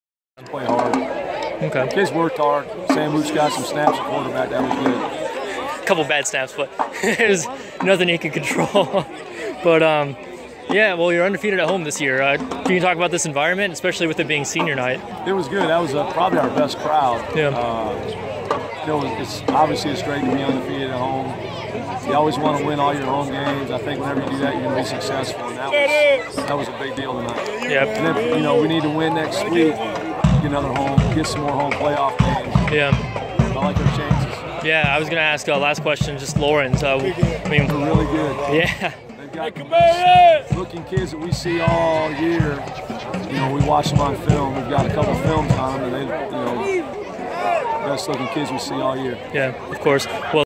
10/21 Dan Pippin after Riverside
Greenwood football coach Dan Pippin talks about the team's 63-14 win over Riverside Friday night.